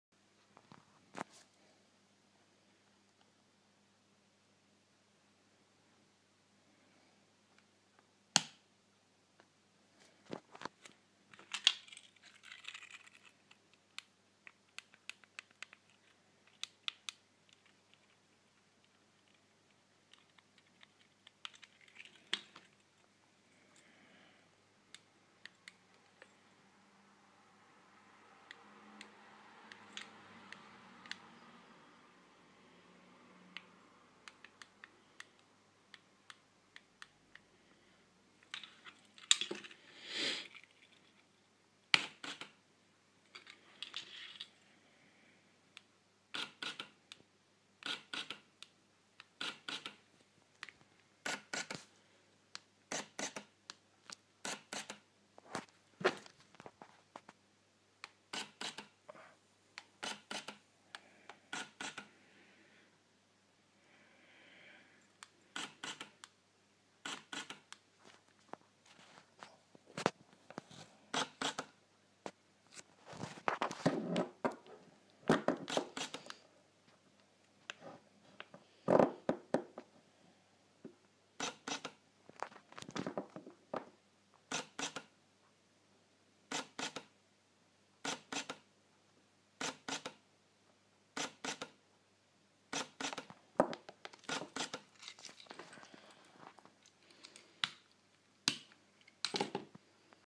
DIY lichtschranke - Selbstauslöser DSLR für Fledermäuse
Ich habe jetzt einige Teile vom Conrad bekommen und habe gleich mal ein Prototyp meiner LowCost DIY Lichtschranke gebaut .